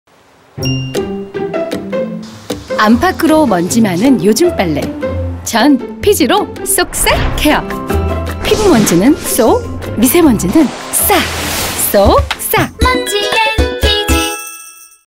미세먼지까지 세탁하는 세탁세제! 새로워진 FiJi 파워젤 TVC
안에서는 피부먼지, 밖에서는 미세먼지
안팎으로 먼지 걱정 많은 요즘 빨래는 FiJi 파워젤로 한 번에 세탁하세요.
7종효소와 더 작아진 미세거품으로 섬유 사이사이 놓치는 때 없이!
이제 피부먼지는 쏙! 미세먼지는 싹!
찌든 때부터 섬유 속 미세먼지까지 FiJi로 한 번에 쏙싹! 하세요